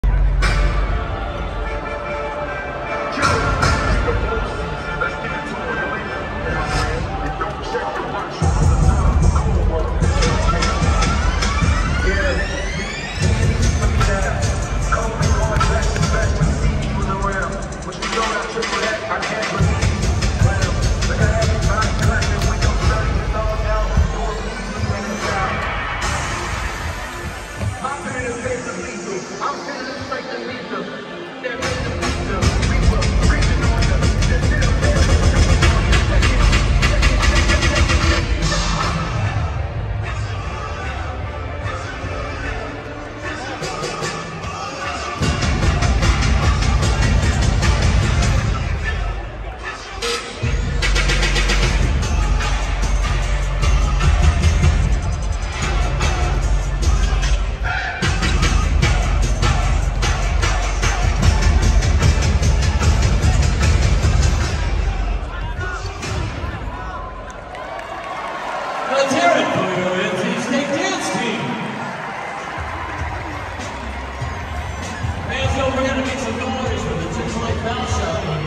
NC State dance team performs - 1.19.22
NC State dance team performs during a timeout in the Wolfpack vs Hokies basketball game at the PNC Arena.

Video by Gene Galin for the Chatham Journal newspaper and ACCBasketballNews.com